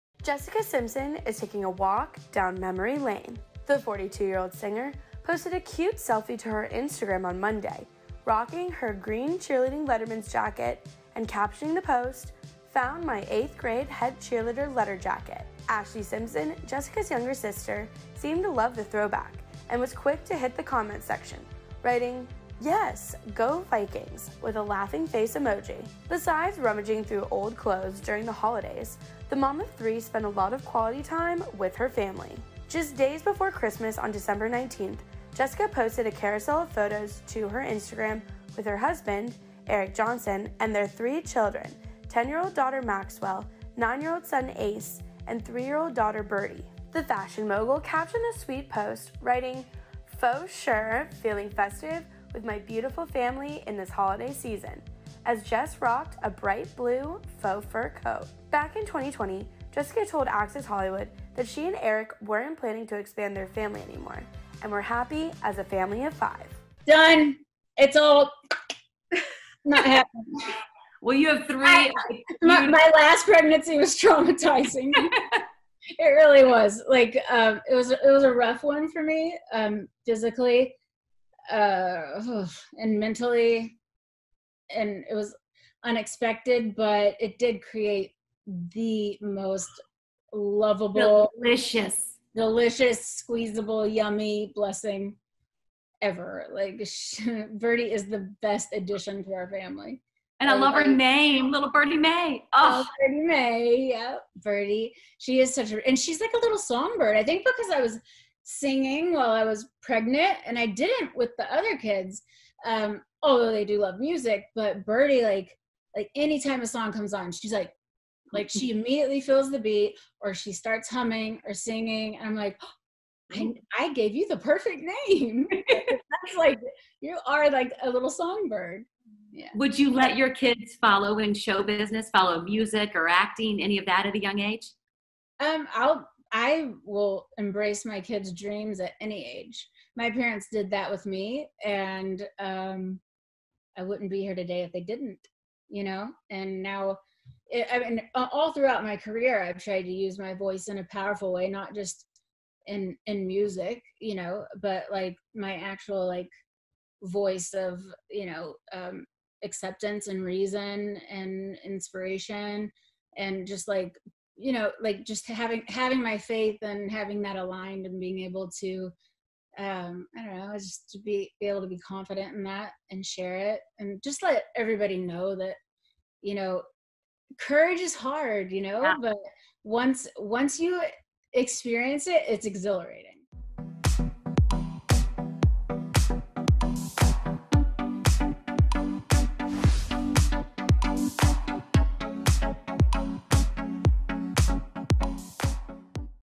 Jessica Simpson Still Fits Into 8th Grade Cheerleader Jacket
Jessica Simpson is taking a walk down memory lane. The 42-year-old singer posted a cute selfie to her Instagram on Monday rocking a green cheerleading letter jacket. "Found my 8th grade head cheerleader letter jacket," she captioned the post. Ashlee Simpson, Jessica's younger sister, seemed to love the throwback and was quick to hit the comment section. "Yes! Go Vikings," she wrote. Besides rummaging through old clothes during the holidays, the mom of three spent quality time with her family!